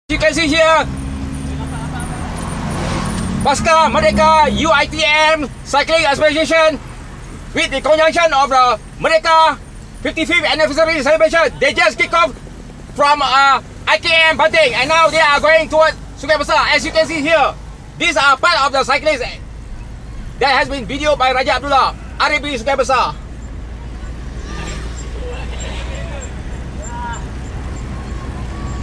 PASCA MERDEKA UITM 1 M'SIA 2012 CYCLING EXPEDITION -SEP1-SEPT 9, 2012
KICKED OFF- UTHM,JOHOR - PERLIS. COVERING 777KM VIDEOGRAPHER:RABSGBESAR CHANNEL